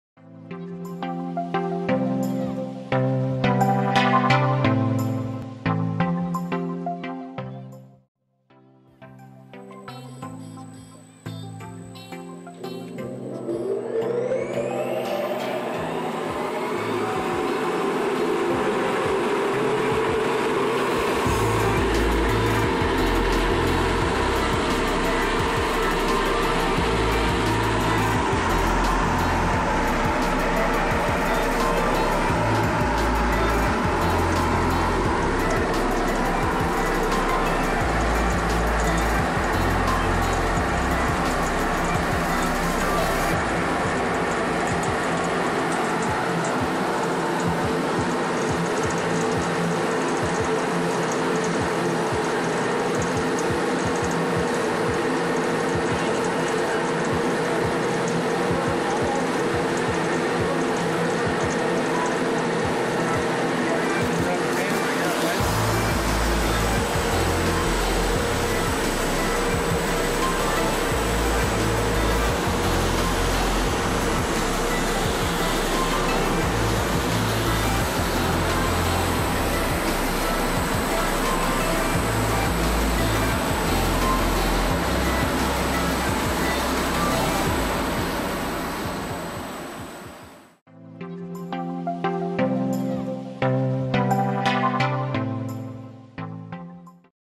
Perfection 2DD-1000 Brush Sander
The video demonstrates brush sanding aluminum parts with profiles such as louvers and tab-up parts. De-burring, edge radius and preparing parts for paint or powder coat. Scuff sand / de-bur aluminum, stainless steel, steel, titanium, composites and plastics. Multiple grits within brush machine for a variety of Ra scratches, non-directional scratch.